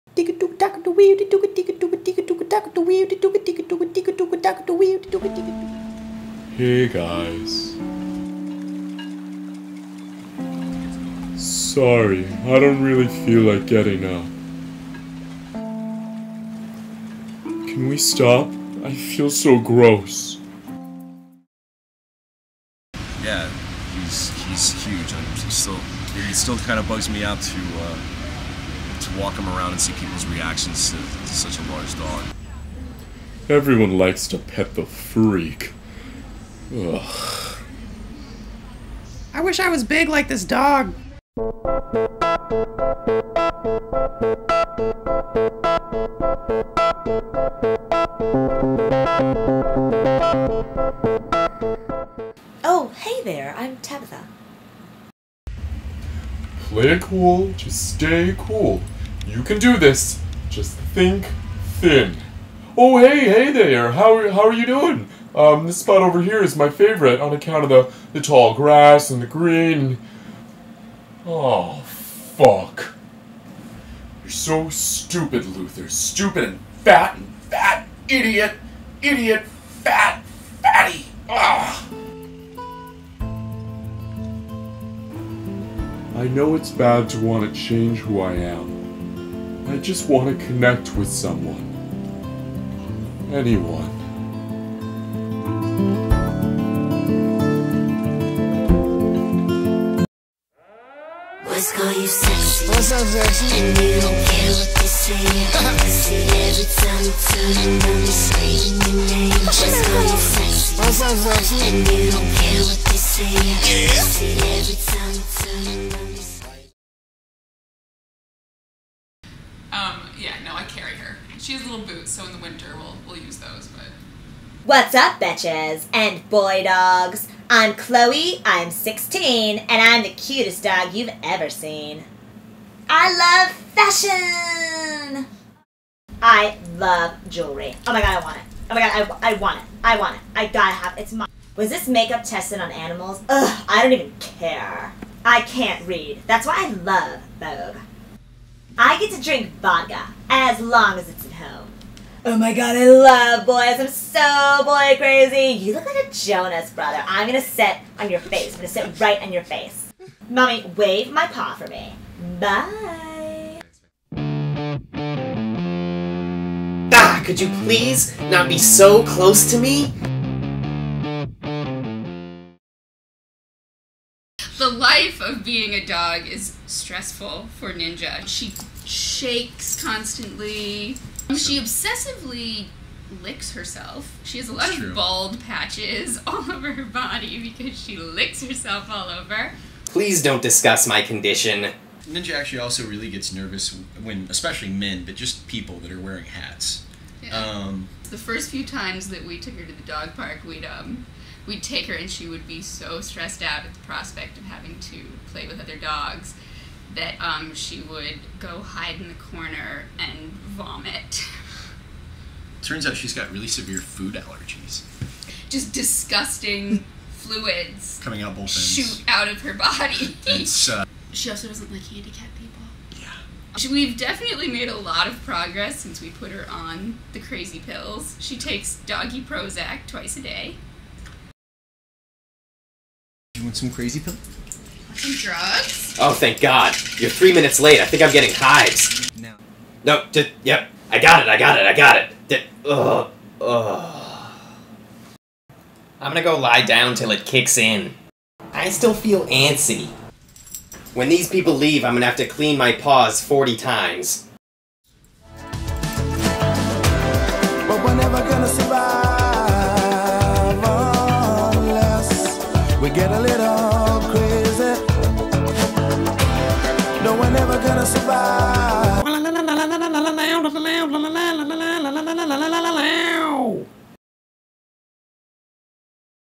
True Life - I'm a Dog in NYC
A documentary (OR SHOULD WE SAY DOGUMENTARY) on what it's really like being a dog in the greatest place to pee on a tree on the planet